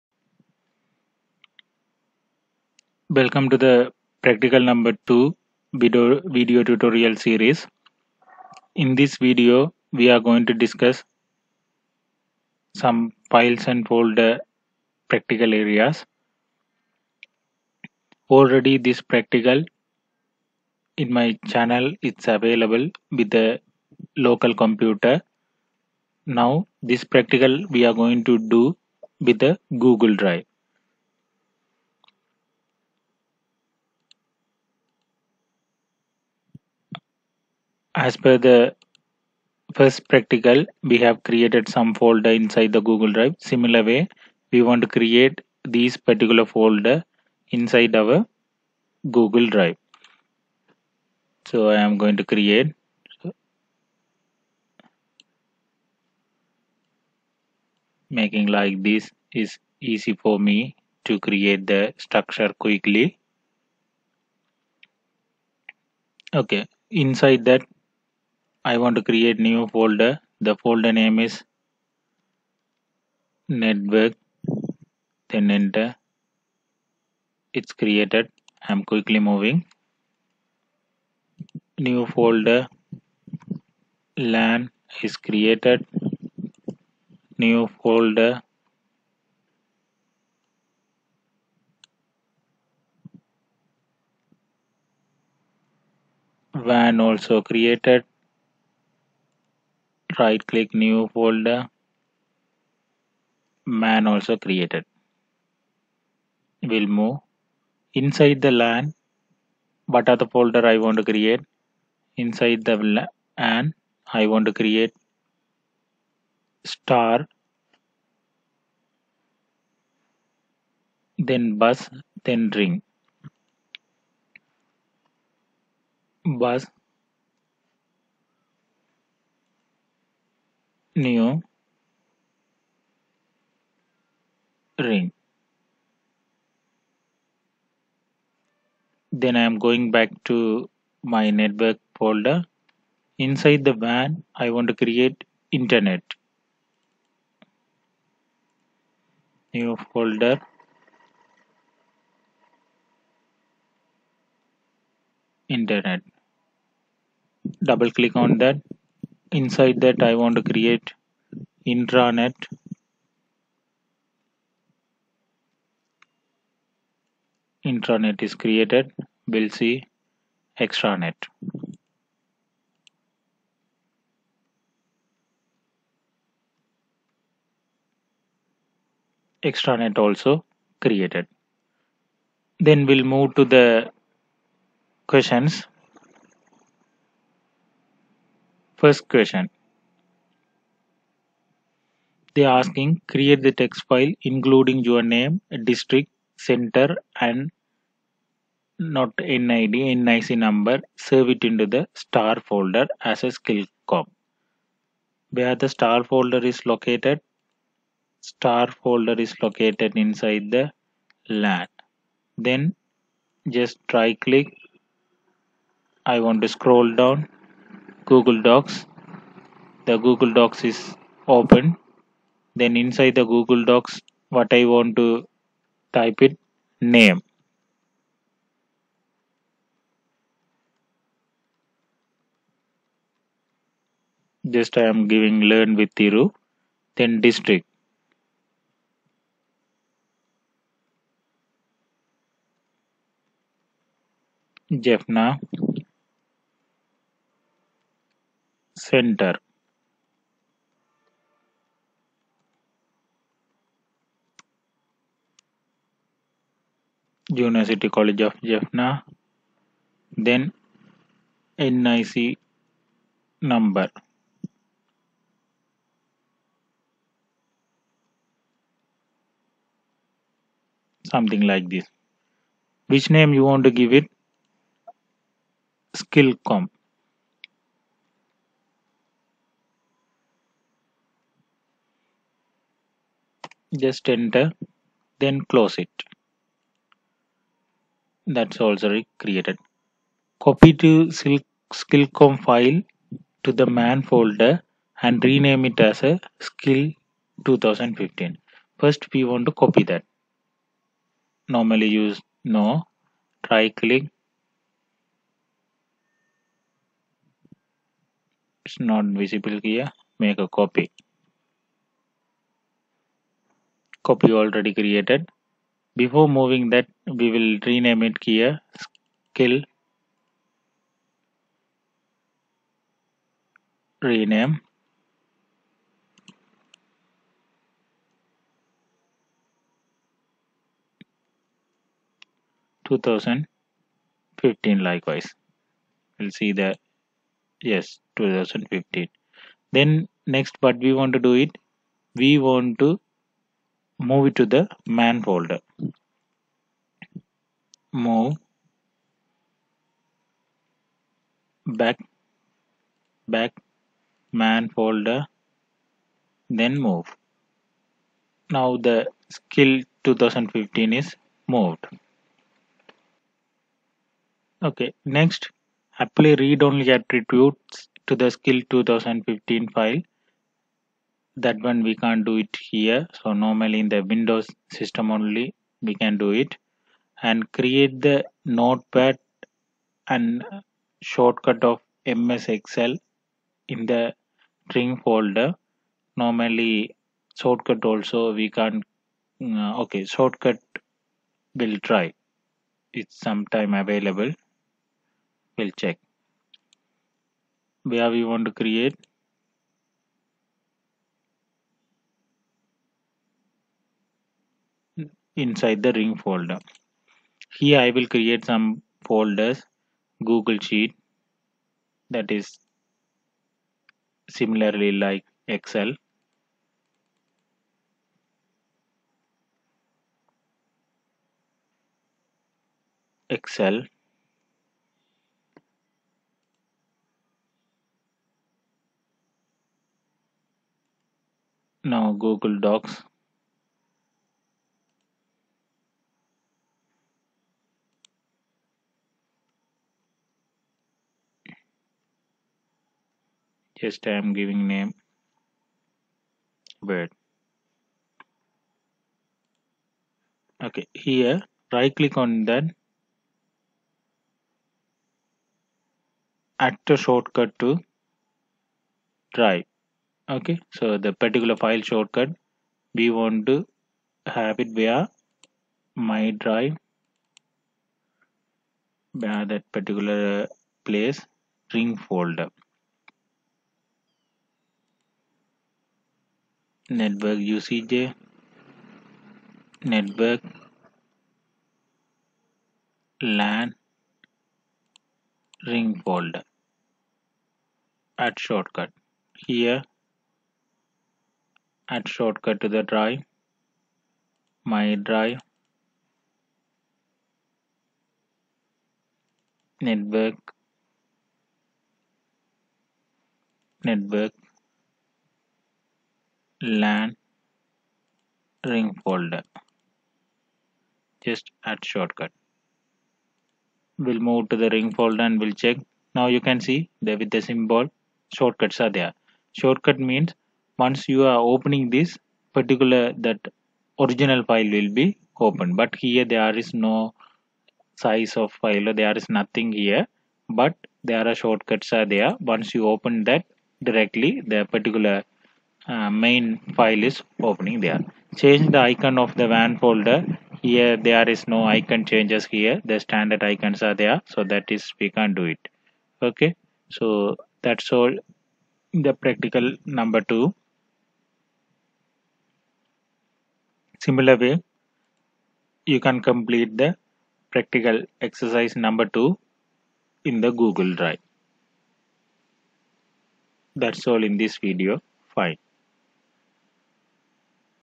Practical 02: Files and Folders Using Google Drive | Computer File Management
Notes:
- Please watch till the end to understand the program.
- Make sure you practice on your own.

And much more available. check out Lear with Thiru's youtube channel.